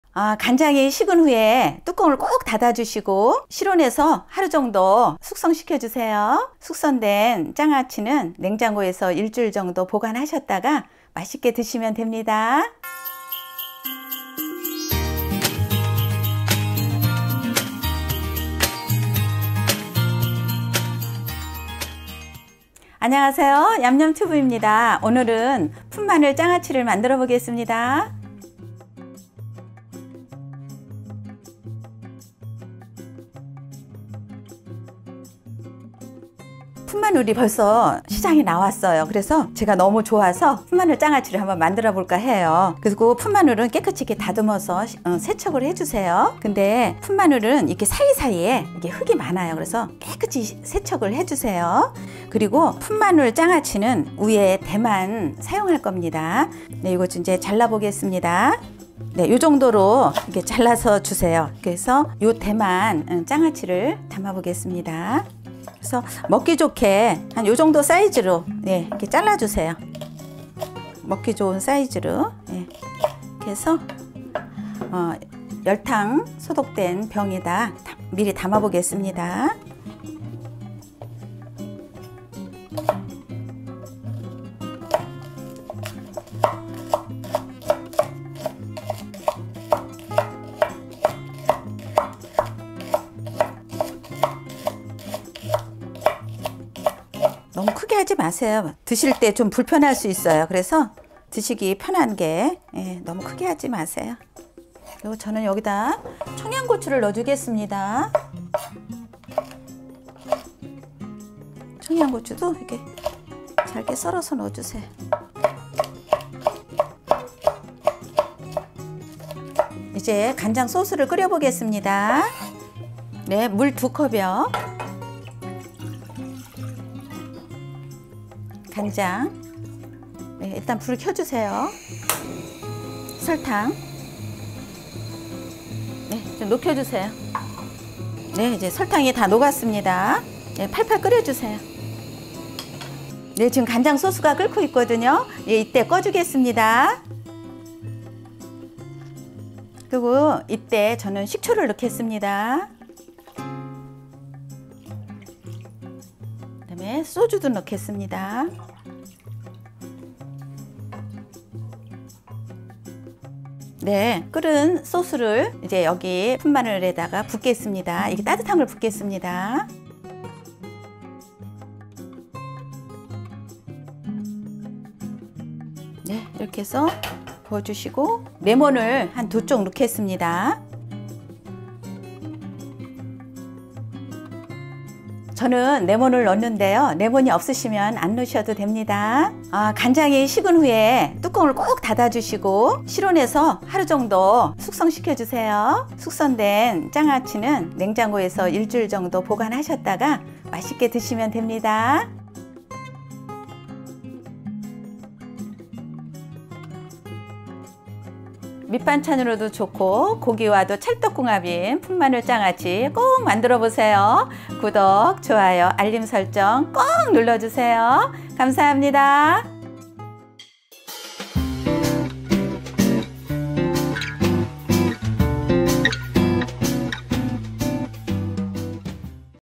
풋마늘장아찌 만드는법 - 간단하게 만드는 풋마늘장아찌
- 풋마늘장아찌
진짜 간단하게 만드는 풋마늘장아찌 레시피

[재료]
풋마늘 500g
간장 1컵
물 2컵
설탕 1/2컵
식초 350ml
소주 1/2컵
레몬 2쪽

- 한식 조리기능사 국가기술자격증
- 중식 조리기능사 국가기술자격증
- 양식 조리기능사 국가기술자격증
- 바리스타 자격증 보유

'언제나 얌얌 먹을수 있는 맛있는 요리만들기'